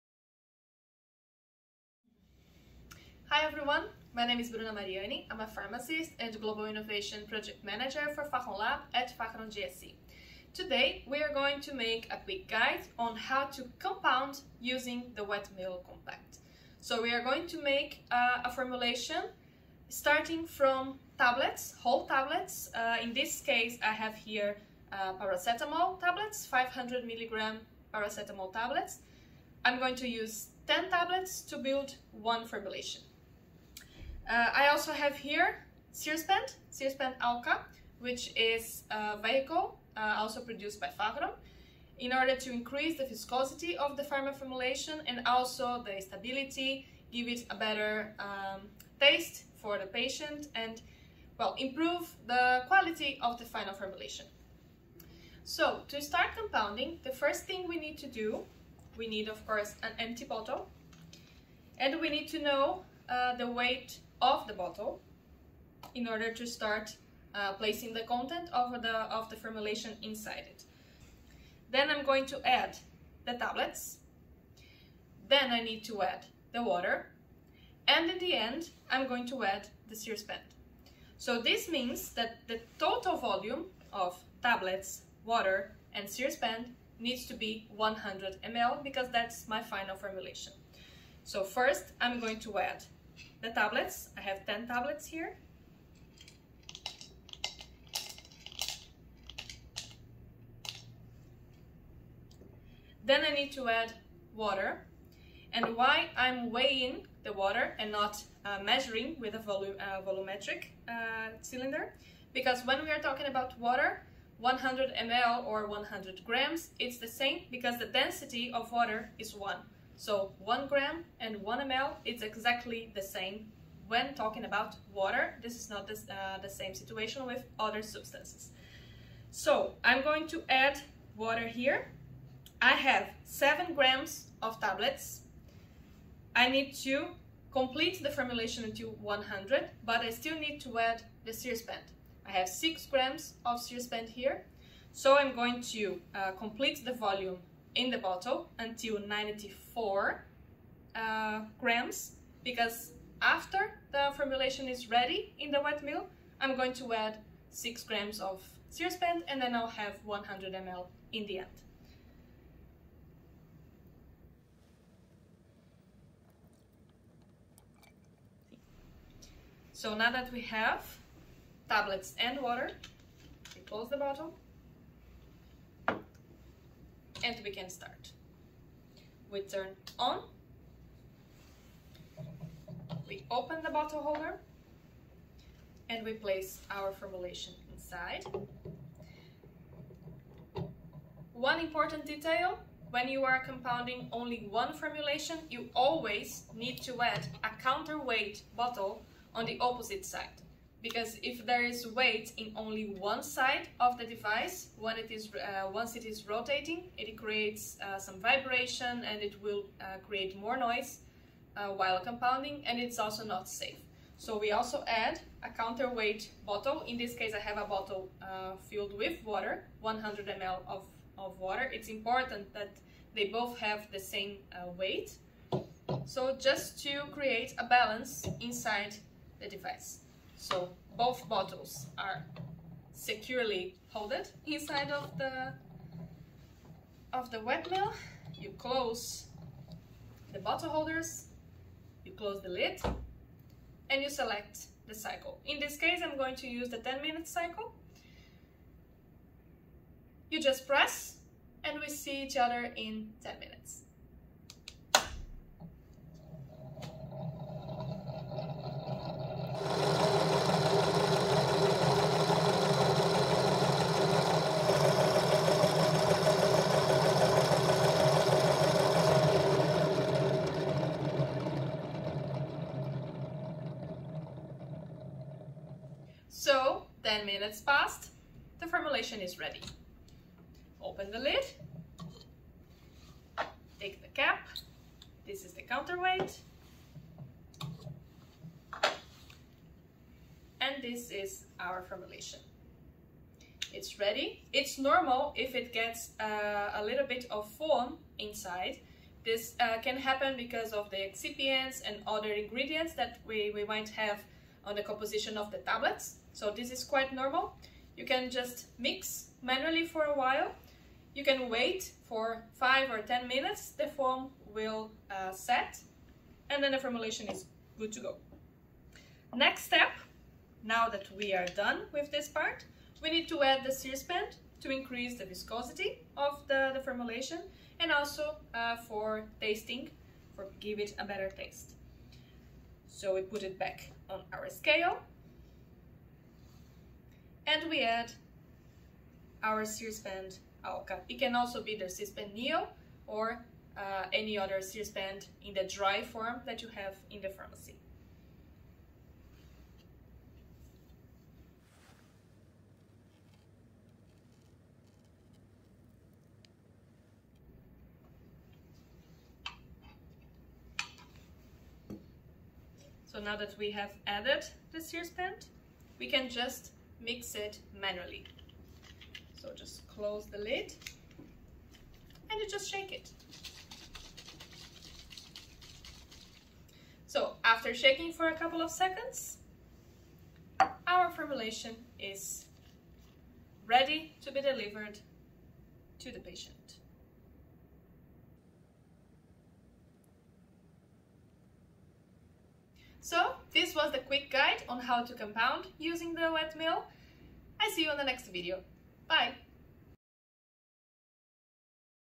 FagronLab WetMill Compact Compounding Guide
Die Compliance einer pharmakologischen Behandlung hängt oft davon ab, ob sie in einer für den Patienten geeigneten Darreichungsform verfügbar ist. Leider erfüllen Fertigarzneimittel nicht immer die spezifischen Bedürfnisse bestimmter Patientengruppen. Dies betrifft Patienten, die z.B. Schwierigkeiten beim Schlucken von Tabletten und Kapseln haben, oder die ihre Arzneimittel über eine nasogastrale Sonde oder über ein Gastrostoma erhalten müssen.
Für die Erfüllung dieser speziellen Bedürfnisse bietet sich die Herstellung einer personalisierten Rezeptur für den Patienten an, um aus der festen Arzneiform eine orale Suspension herzustellen.

Bitte beachten Sie, dass die verwendeten Rezepturbestandteile in diesem Video nur der Veranschaulichung dienen und keine korrekte Rezeptur darstellen.